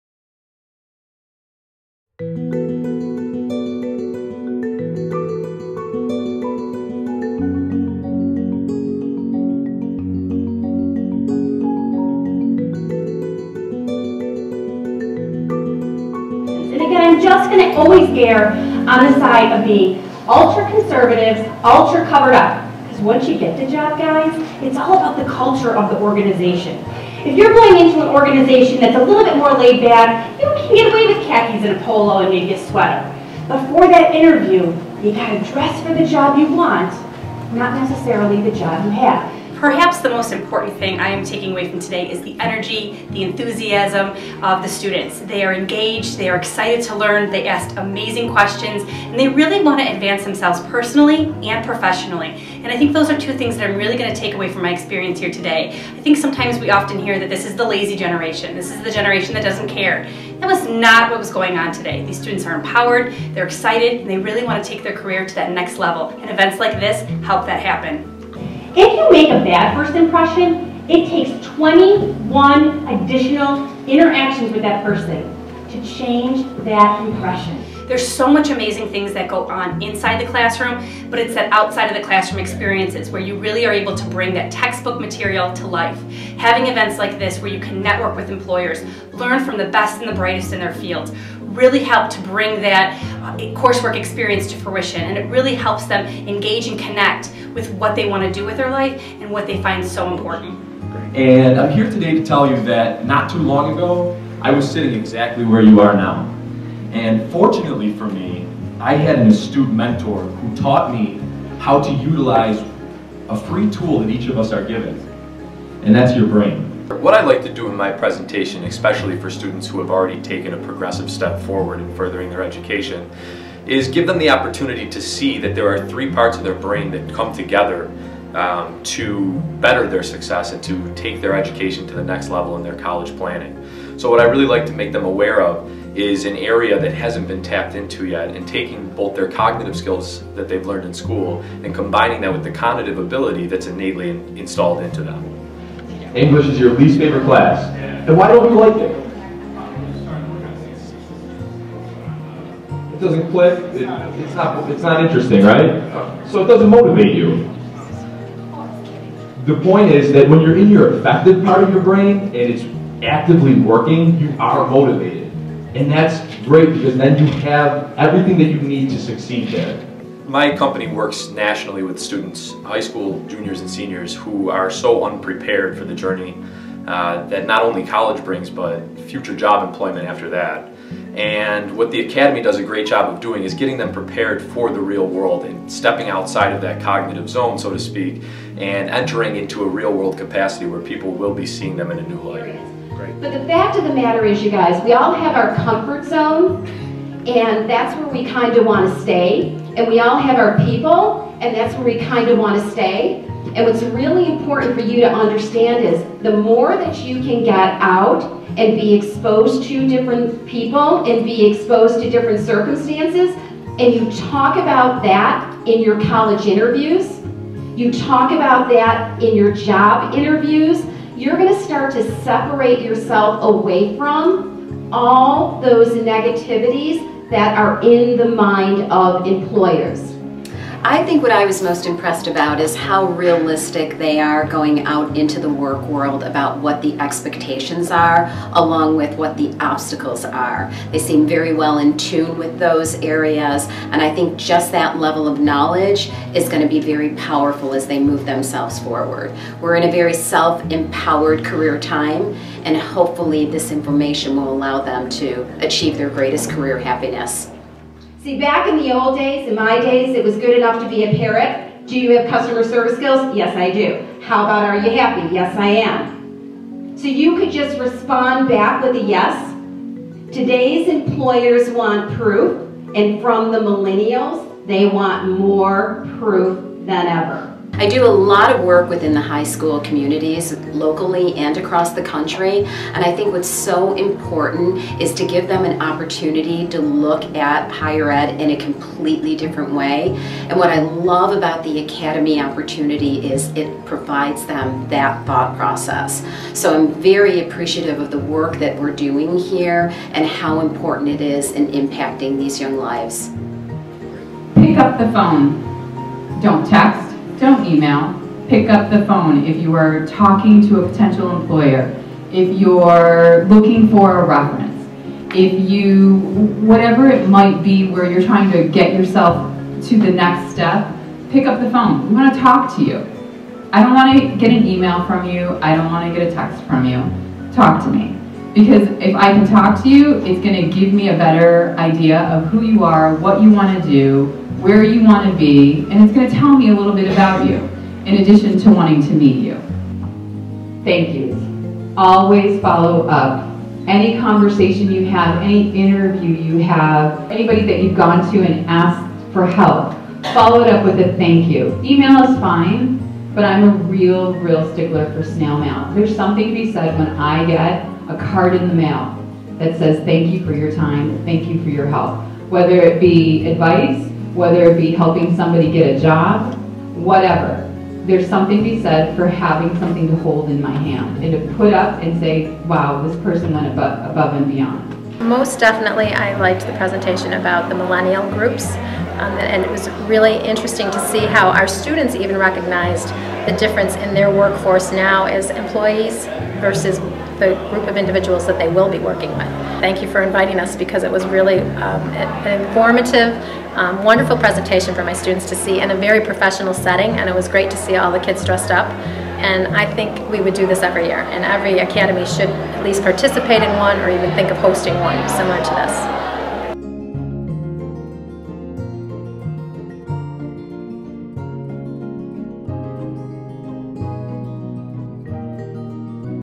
Marketing Me ! 2014 Recap
This video highlights the 2014 student conference hosted by the CTE and Academy Programs here at West Seneca Schools.  Four sessions gave area students the opportunity to hear from various professionals on how to market themselves.